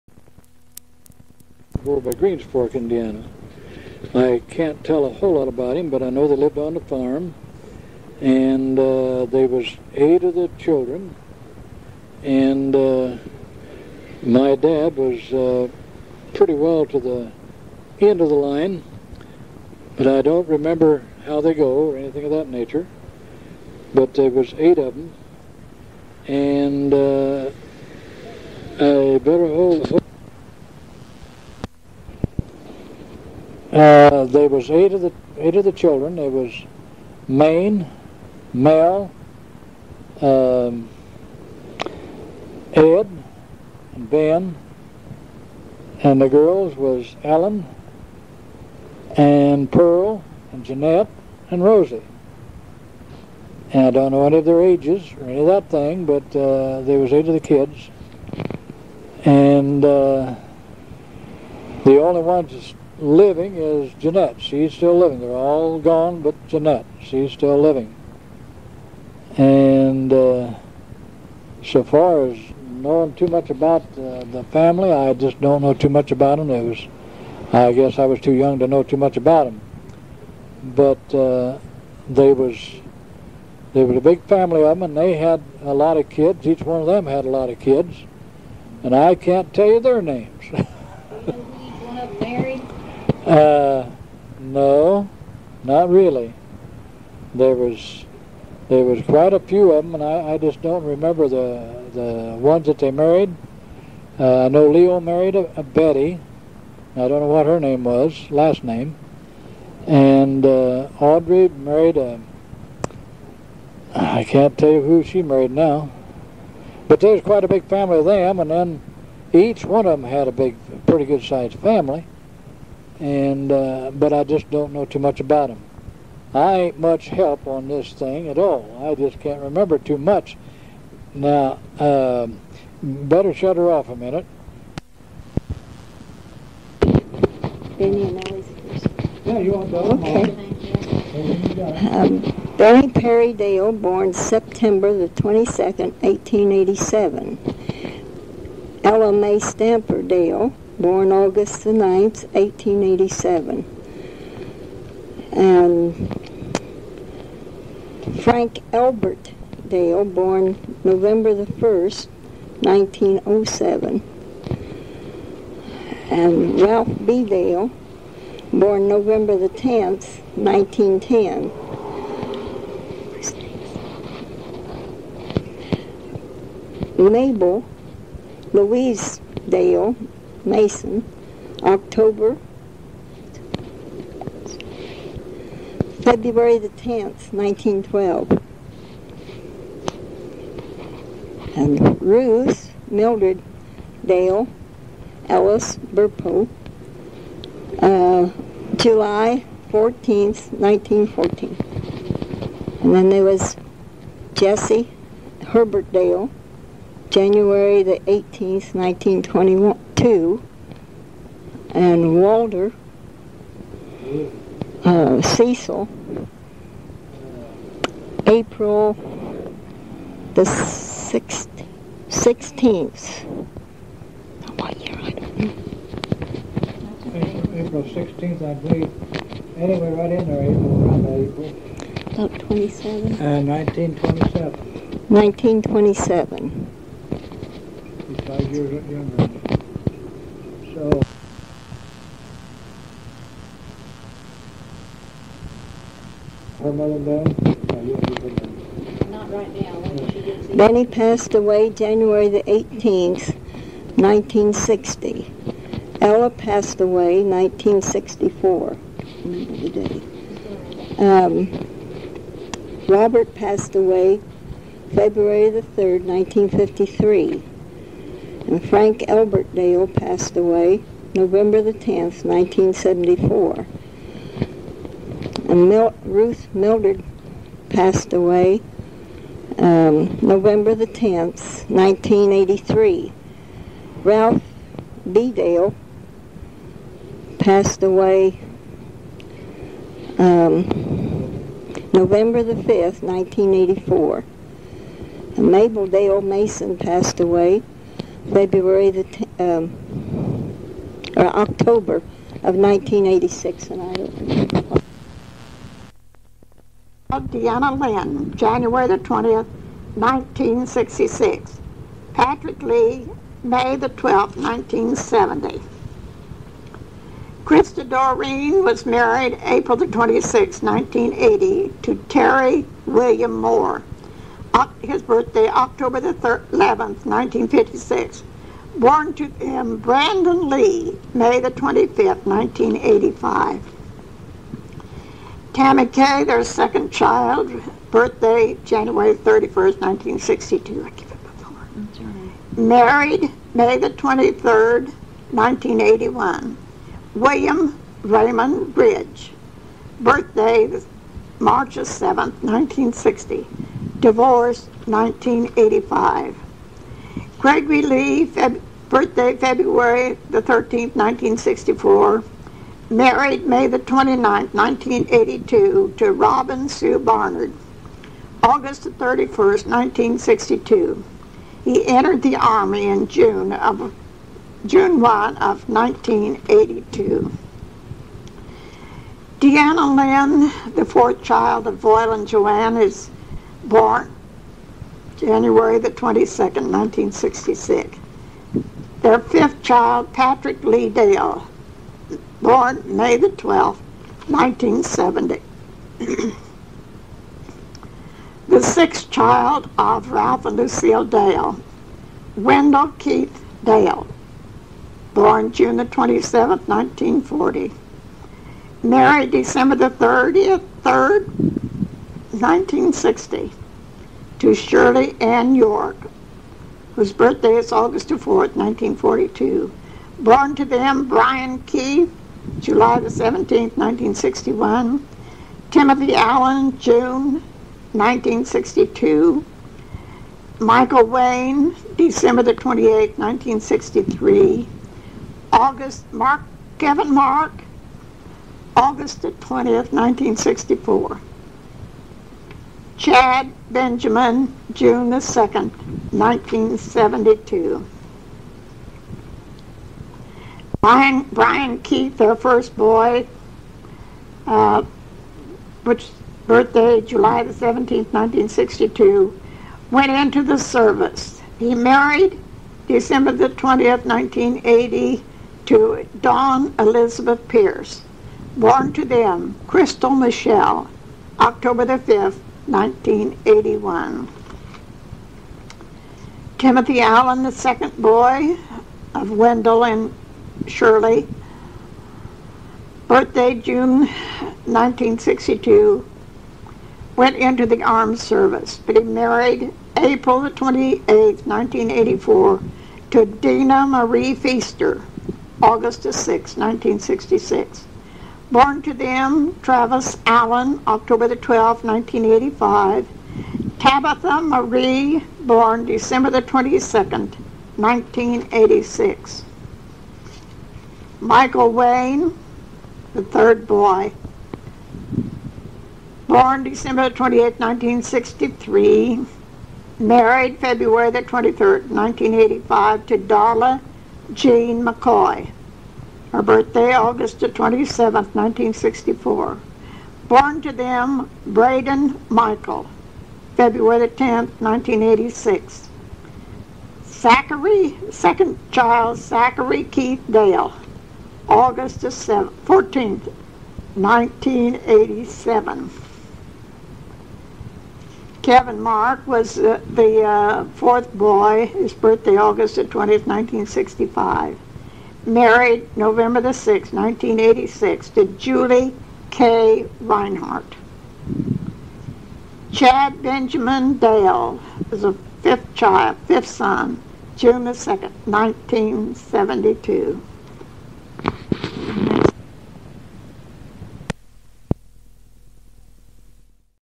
Jesse and Irene Dale family history with Gay Smith
Jesse Dale is my Grandfather Claiborn's 1st cousin. His father, George, is the brother of my Great Grandmother Cora Dale Smith. Jesse is the brother of Ralph Dale ... who is the father of Wendell "Winky" Dale ... who is the father of Bryan, Timmy, Mike, Kevin and Chad Dale. These fellas are my 3rd cousins.